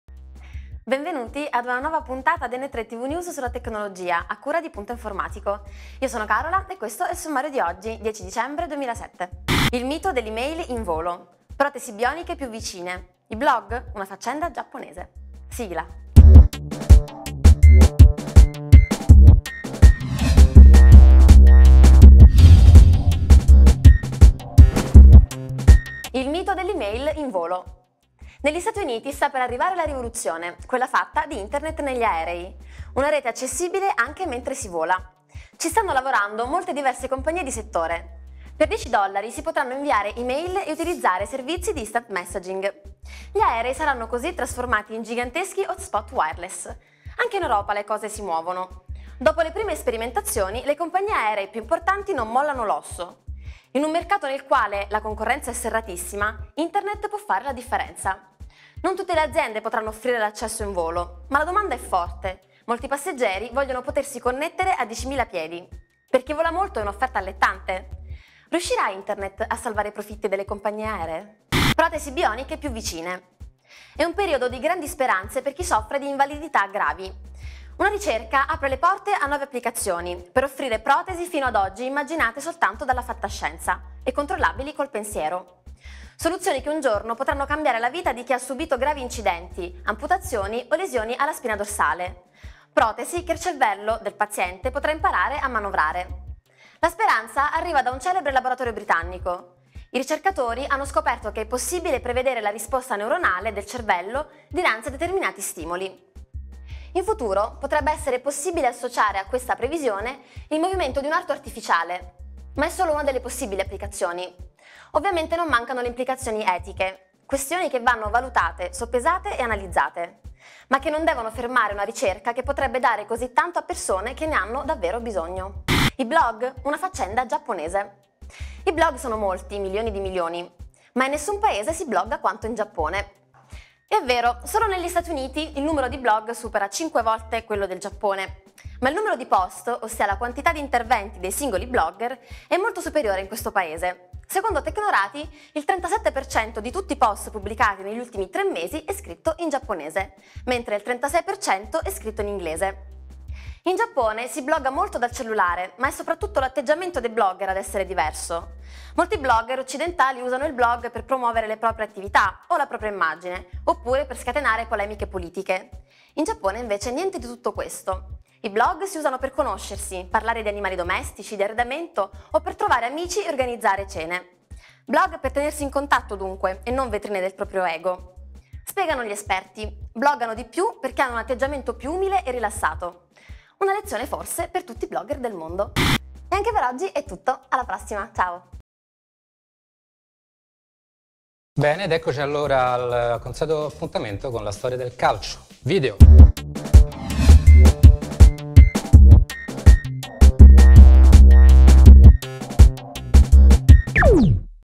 N3TV TechNiuz: dailynews dal mondo geek 10.12.07
- IL MITO DELL'EMAIL IN VOLO
- PROTESI BIONICHE PIU' VICINE
- I BLOG? UNA FACCENDA GIAPPONESE